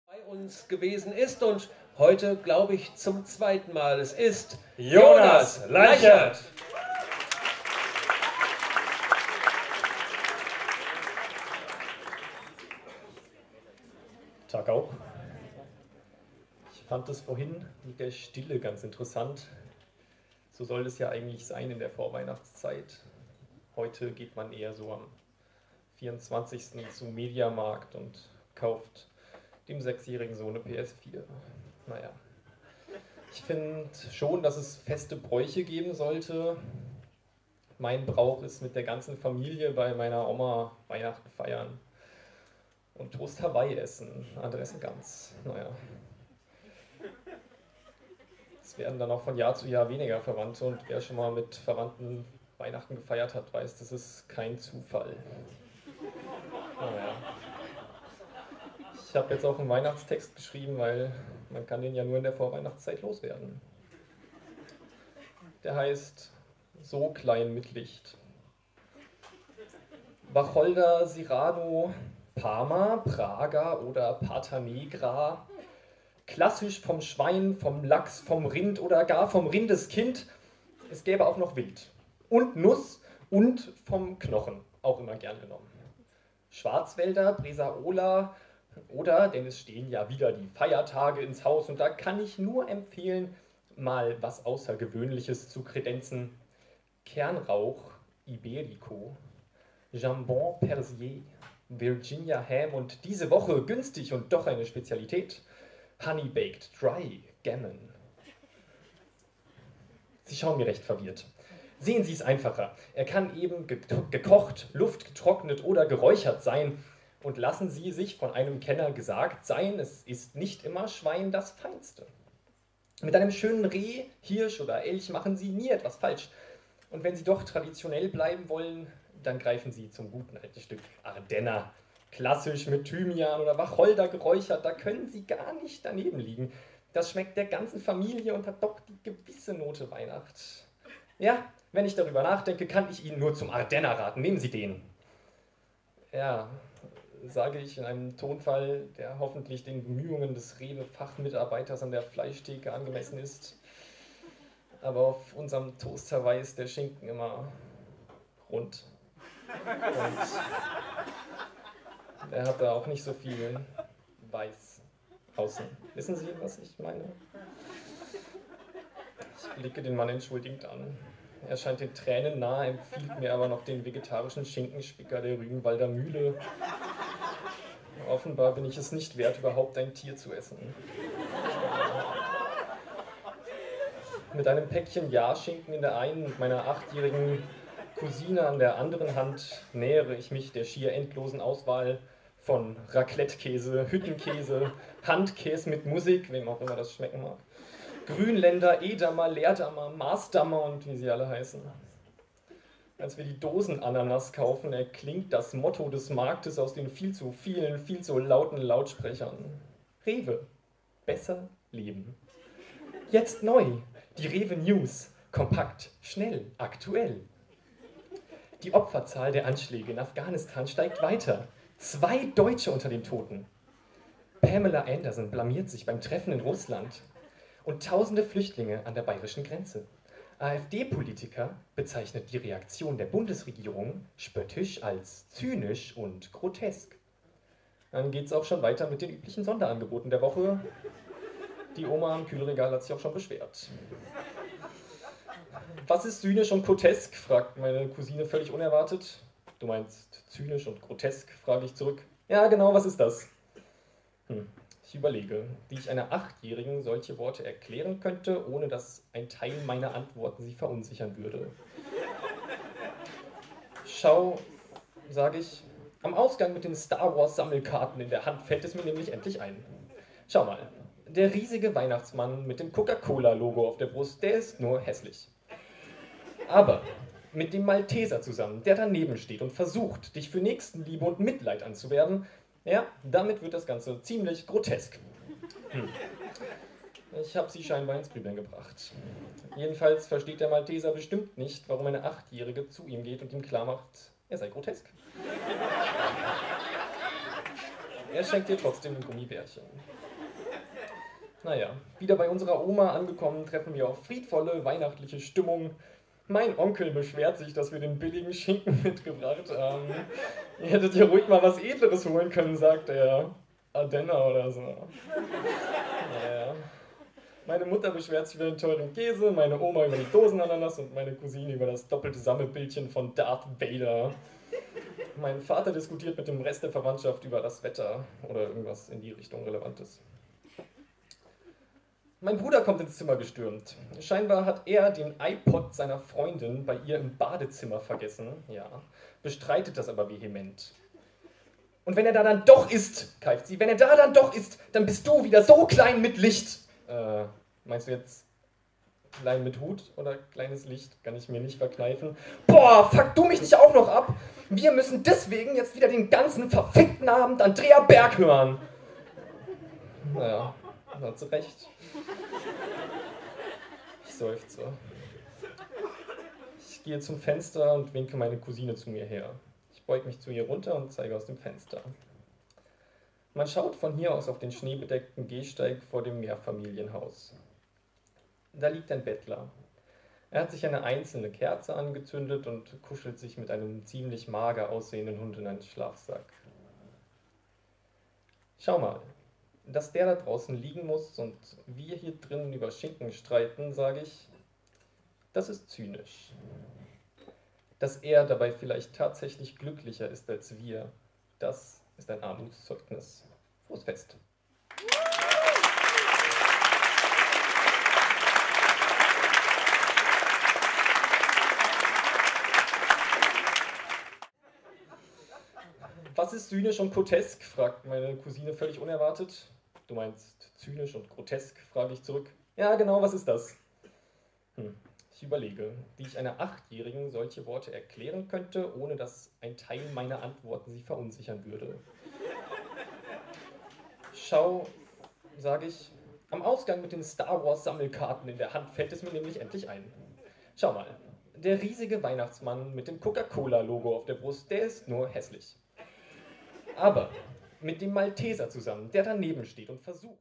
Jonas Leichert - So klein mit Licht
Jonas Leichert war Sieger des Poetry Slam Frankfurt am 11. Dezember 2015.